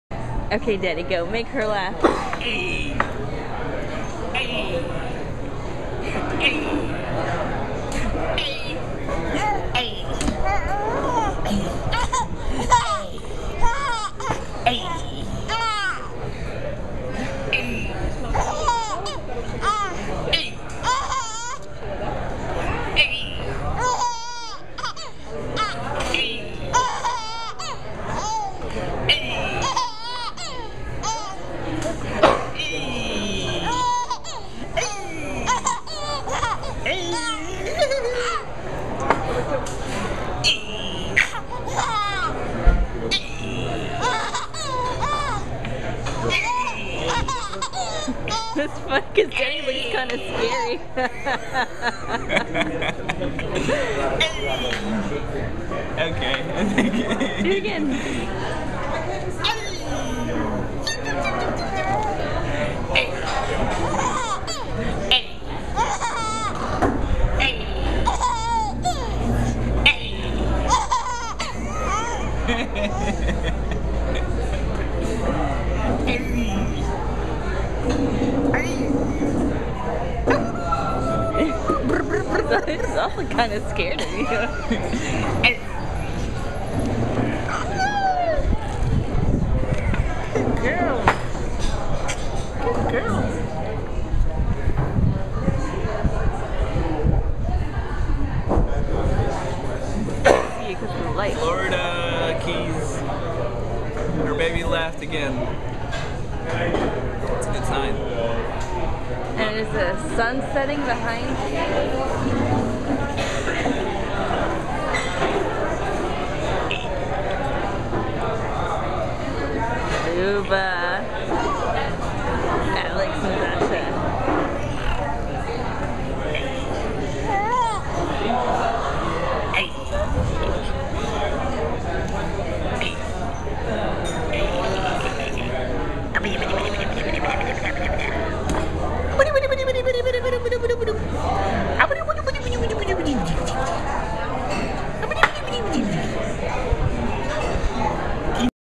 Small Baby Laughing at Daddy
Estelle laughing at daddy. Florida Keys.